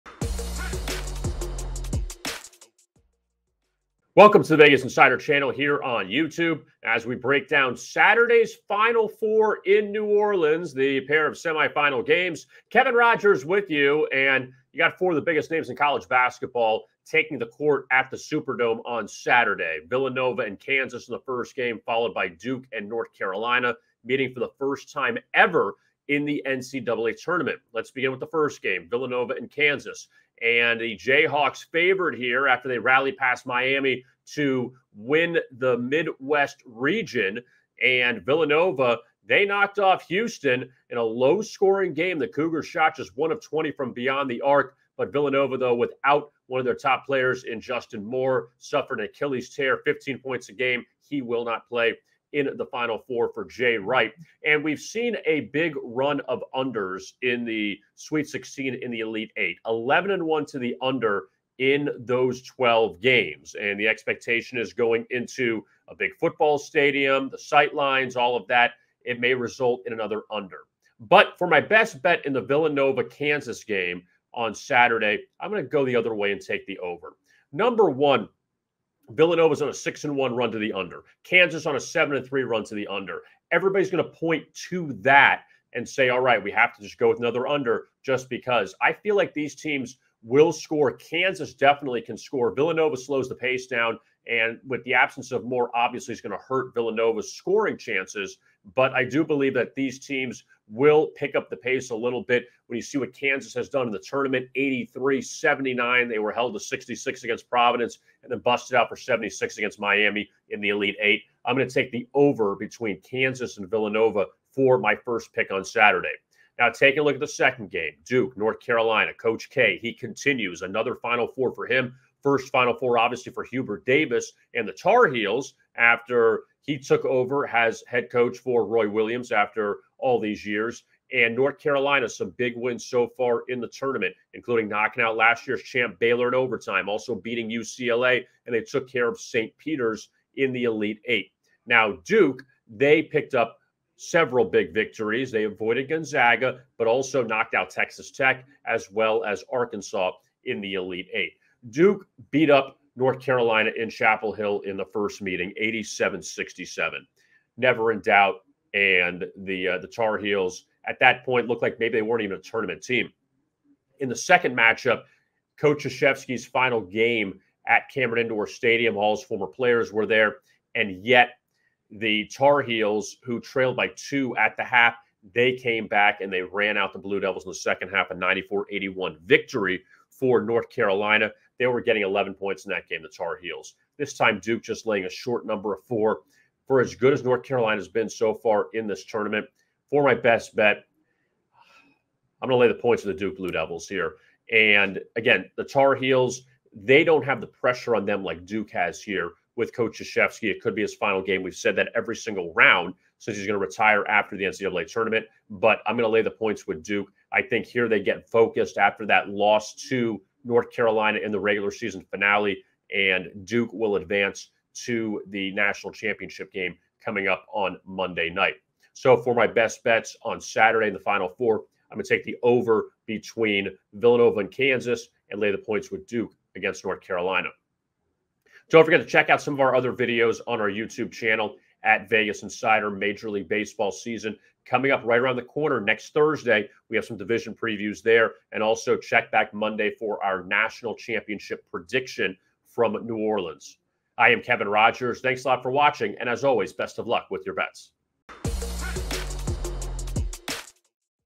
Saturday's March Madness NCAA Tournament Predictions: Final Four Odds and Best Bets
Vegas Insider expert Kevin Rogers gives NCAA tournament predictions for Saturday's Final Four matchups between Kansas and Villanova, as well as Duke battling rival North Carolina.

Rogers offers his predictions on these matchups that will be nationally televised on Saturday evening.